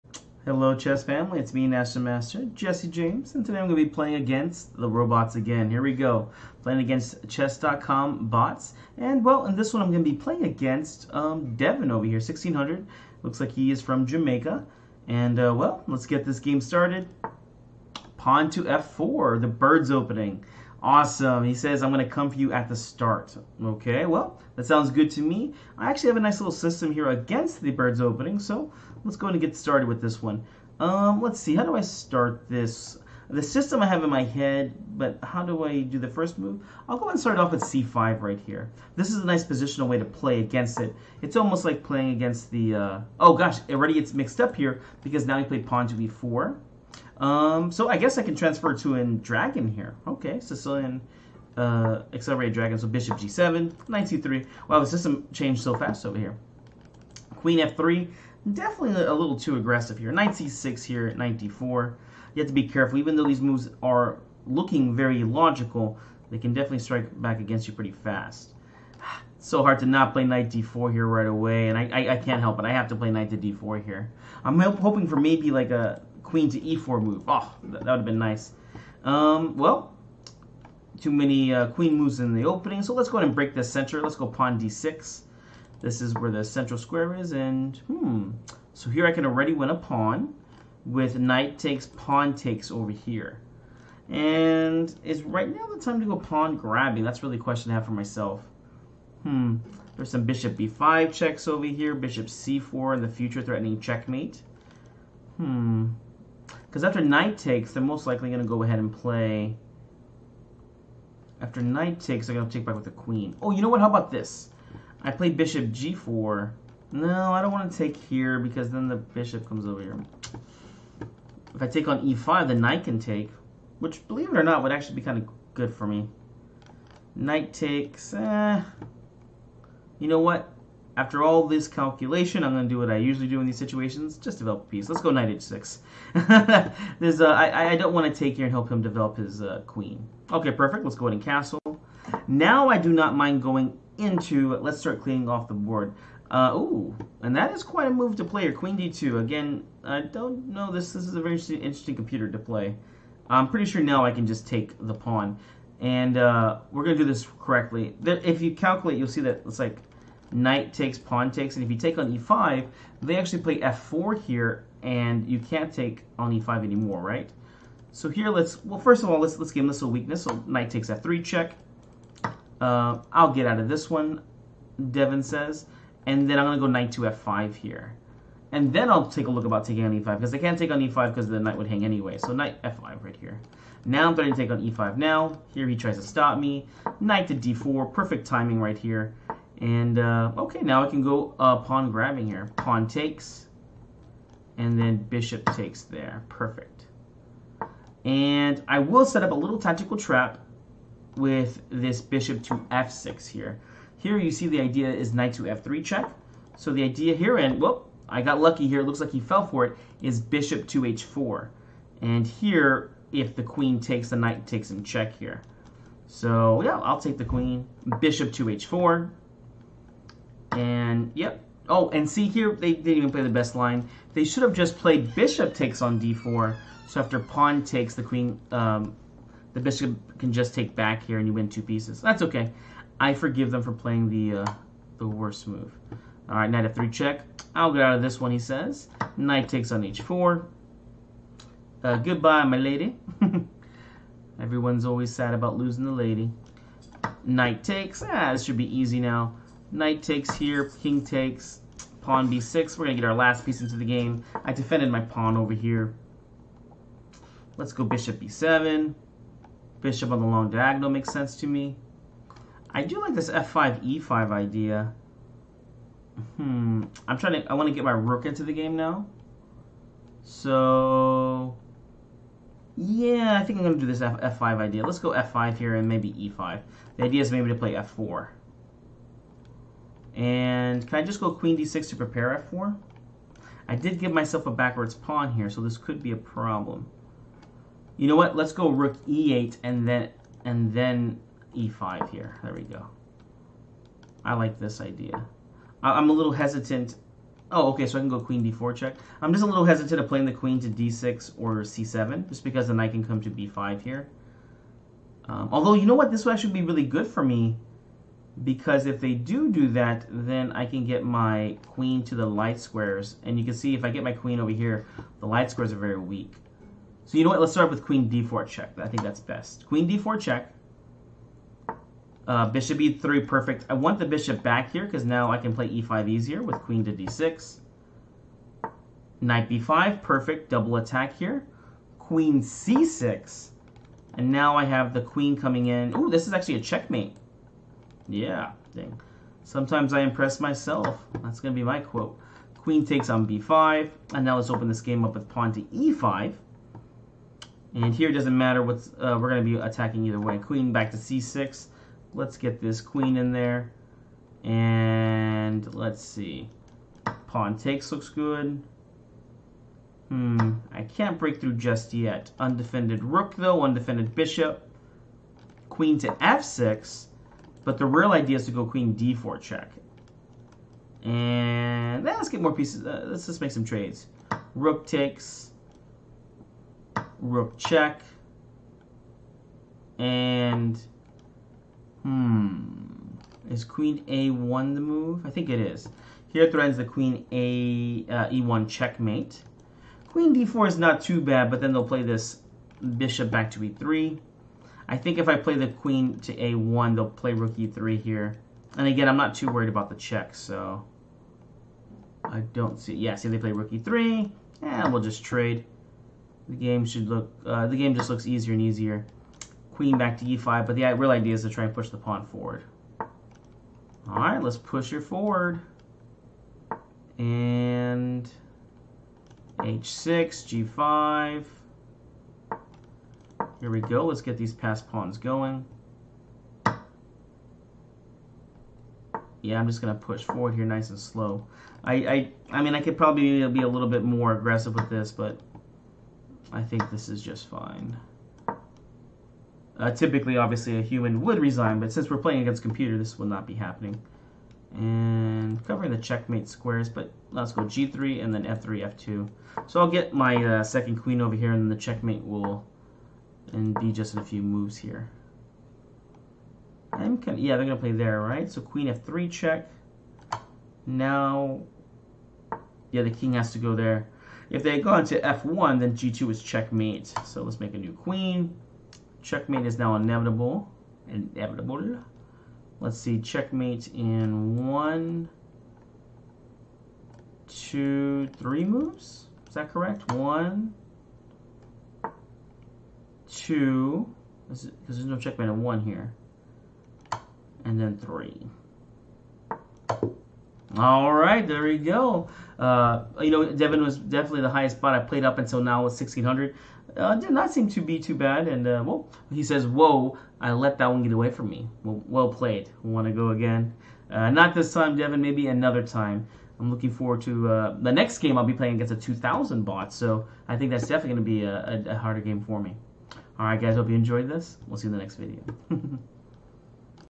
Accelerated Dragon vs Devon
Coach Jesse is taking on the chesscom bots! In this game, Jesse plays the acceleratedragon against Devon and gets a huge advantage in the opening. Master vs Bot, Master wins! Enjoy this fun attacking game.